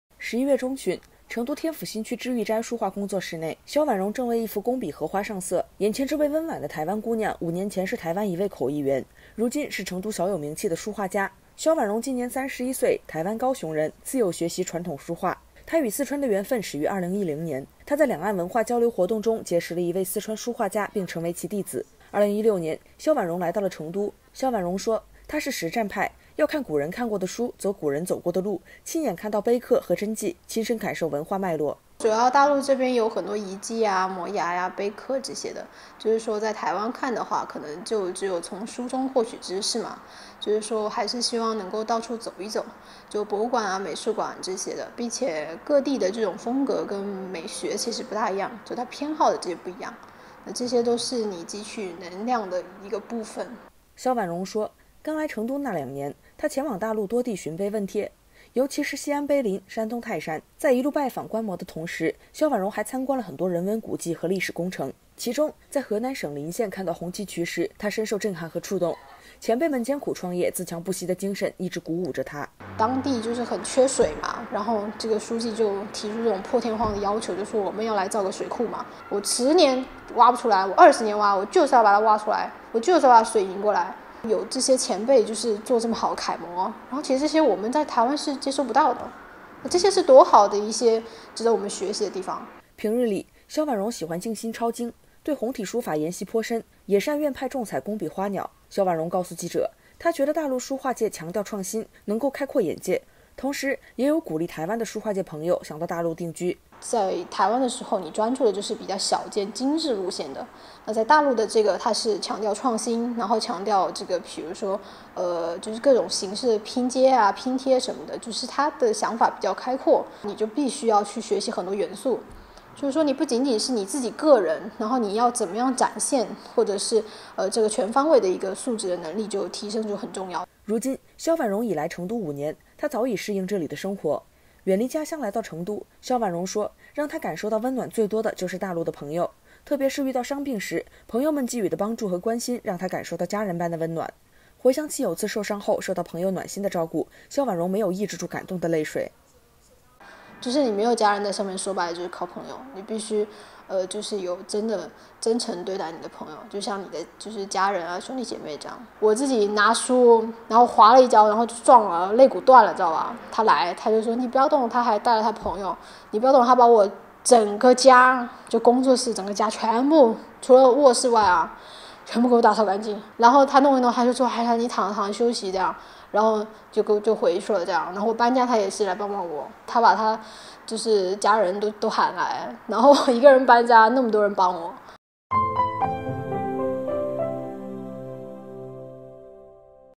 从高雄口译员到成都书画家 台湾姑娘在大陆华丽“转身”
11月中旬，成都天府新区“知遇斋”书画工作室内，萧琬融正为一幅工笔荷花上色。眼前这位温婉的台湾姑娘，五年前是台湾一位口译员，如今是成都小有名气的书画家。